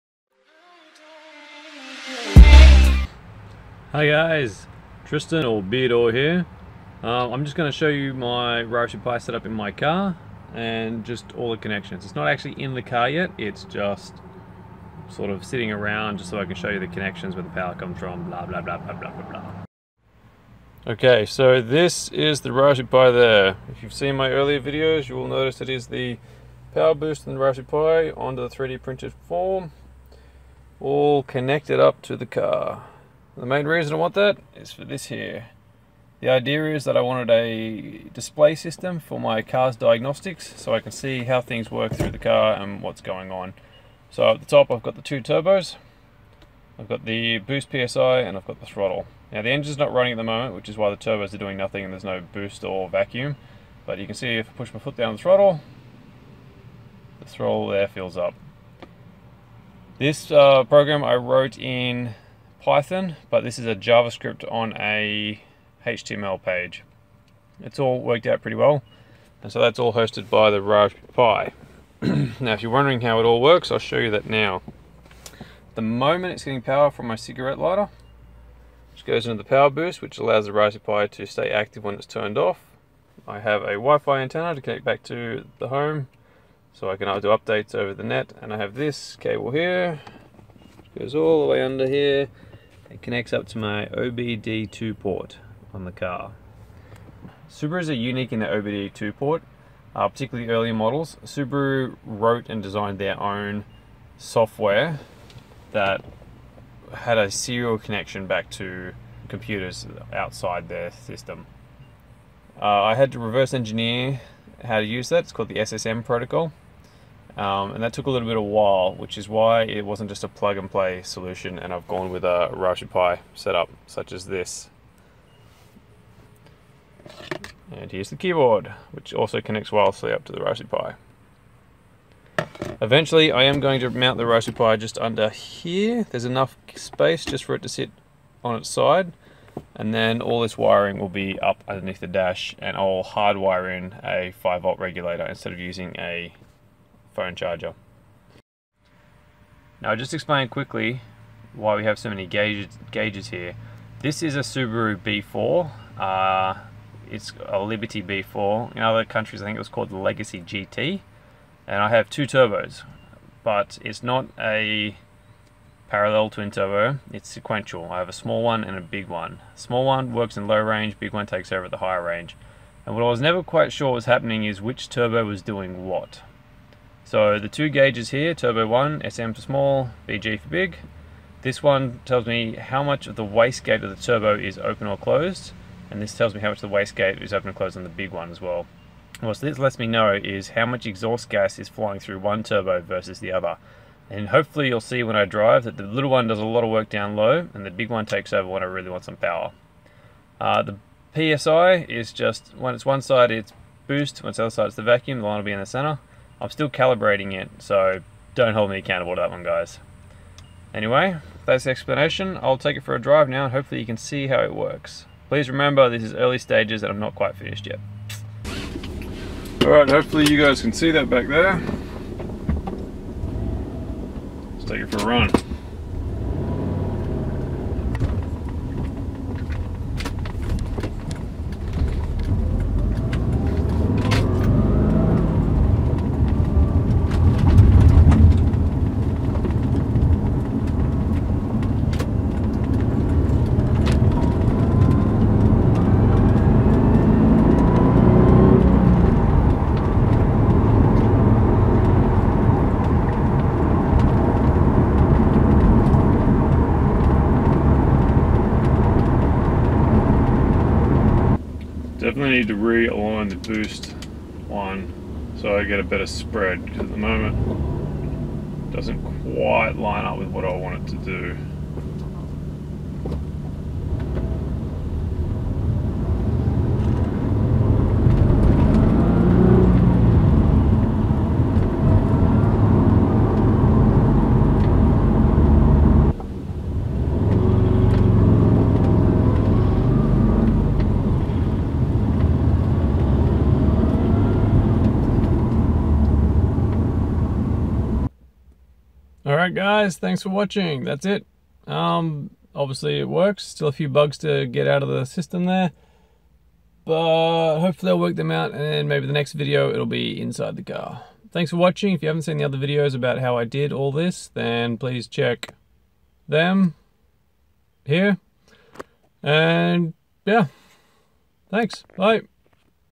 Raspberry Pi digital car gauges display.
Using a raspberry pi and a OBD2 connector I display car diagnostic information on my DVD player.
The program was written in Python 2.7, using a HTML library for the display. The graphics are a JavaScript inside the HTML.
This is hosted on a Raspberry Pi with a PowerBoost 1000c.